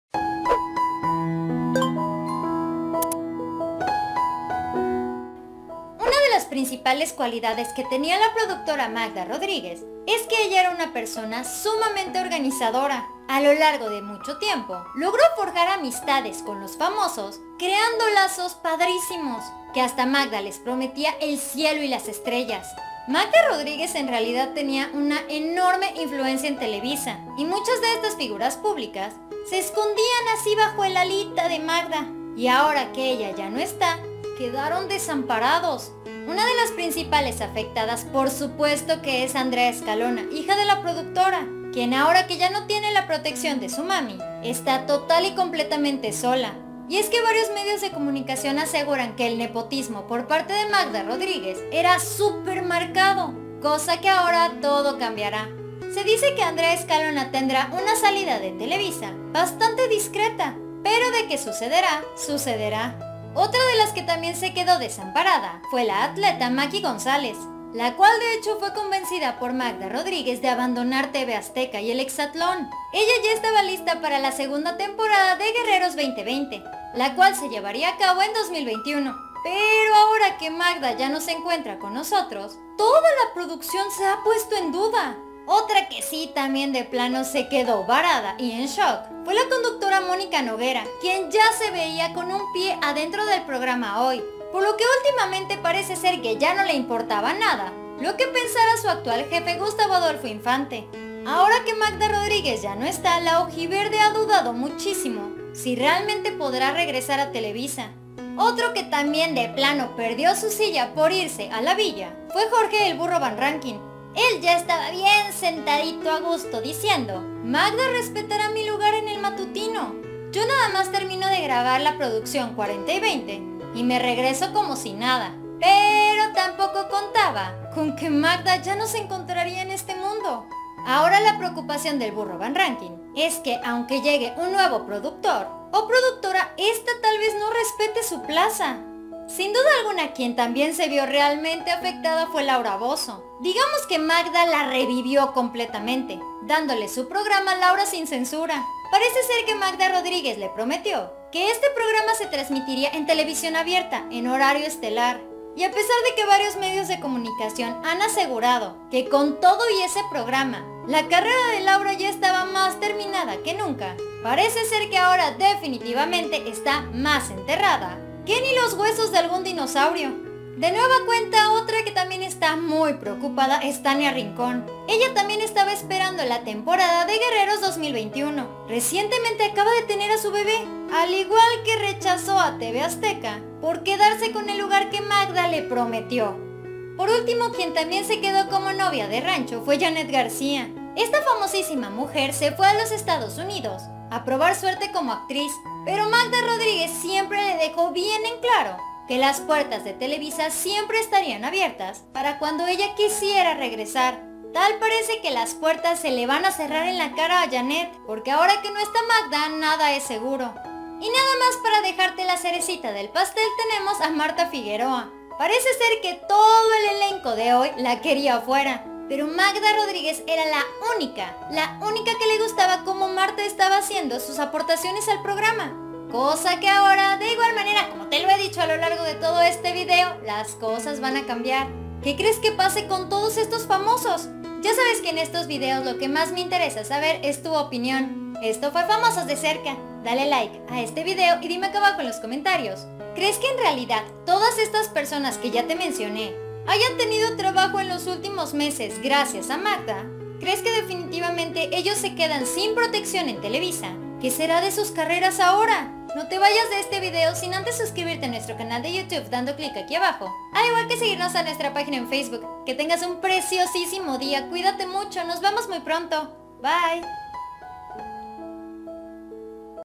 😭🖤 ¡ En Ruinas ! 🖤 ¡ 𝗙𝗮𝗺𝗼𝘀𝗼𝘀 𝗦𝗘 𝗛𝗨𝗡𝗗𝗘𝗡 Tras La Partida de 𝗠𝗮𝗴𝗱𝗮 𝗥𝗼𝗱𝗿𝗶𝗴𝘂𝗲𝘇 ! 😭
A esto se enfrentán estos FAMOSOS con la ausencia de Magda Rodriguez.

 BIENVENIDOS!!!